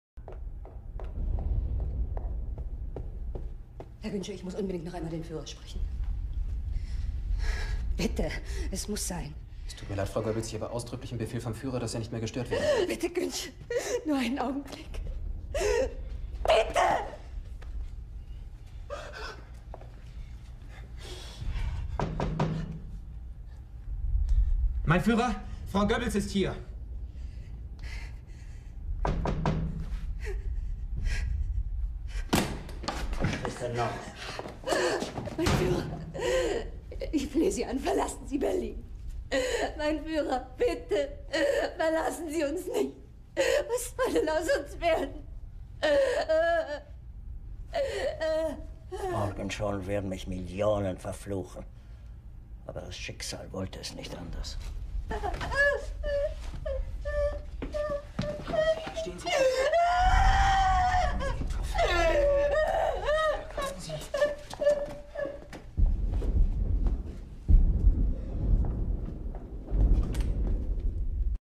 Hitler tells Magda the Tampa Bay Lightning are the 2021 Stanley Cup Champions
The shortened season comes to an end, and the NHL on NBC signs off for the last time.